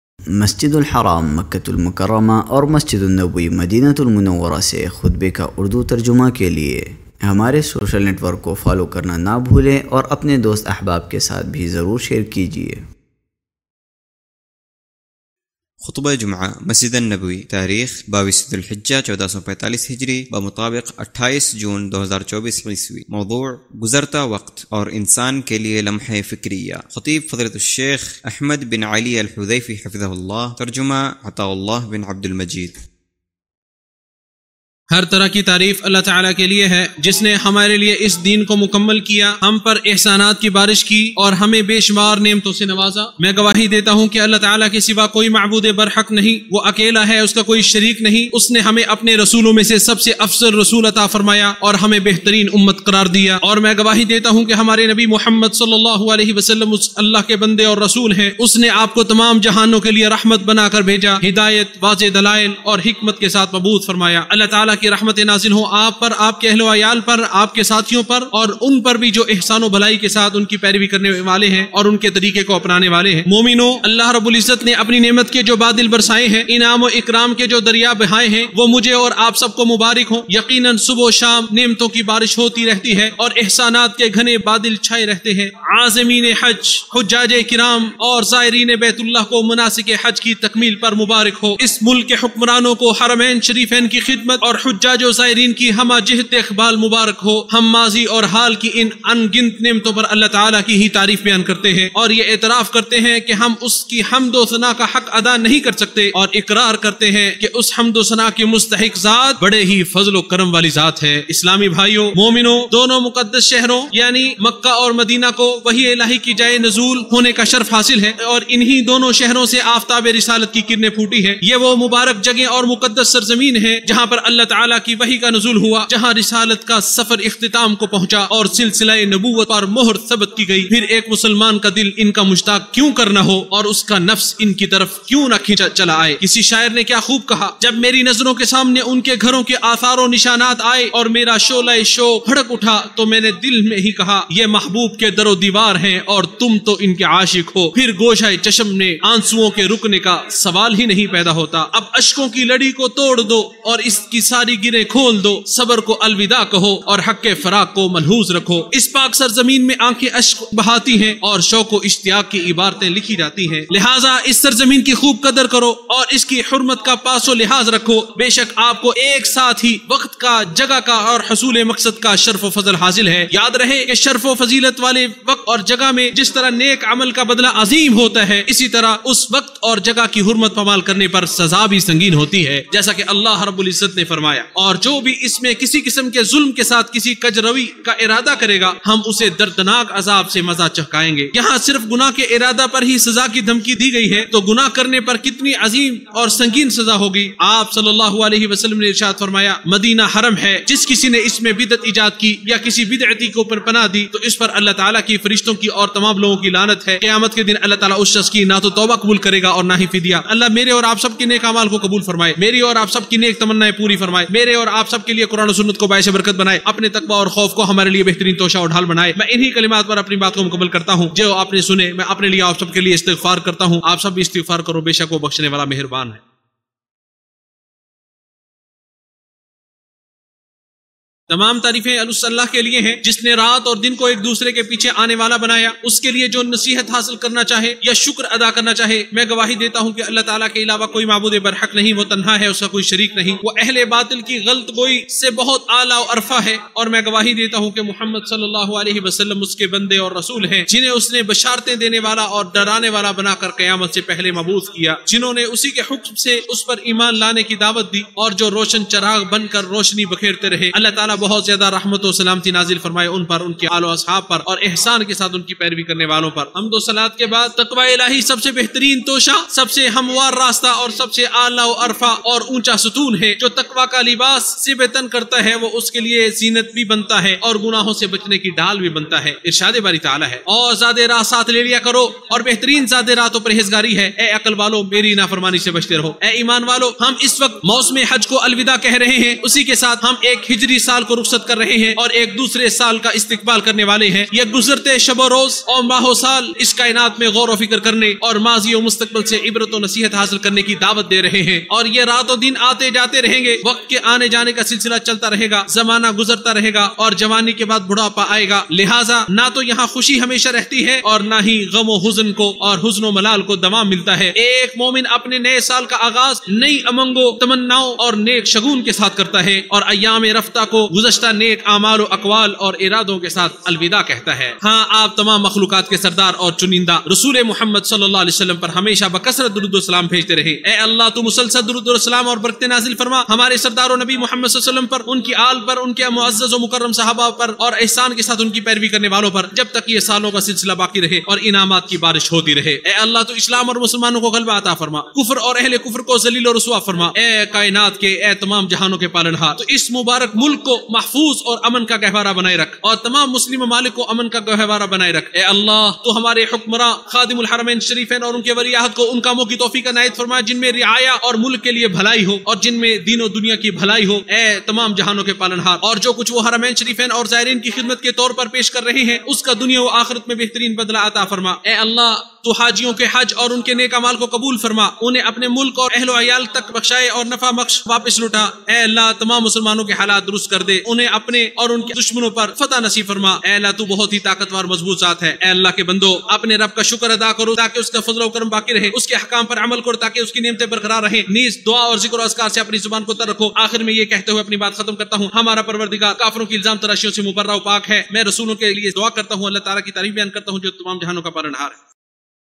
Khutbah-Madinah 28-Jun-24 | Guzarta Waqt Aur Insaan Keliye Lamhae Fikreeyah
گزرتا وقت اور انسان کے لیے لمحہ فکریہ

Weekly Translation of Haramain Khutbah in Urdu-Hindi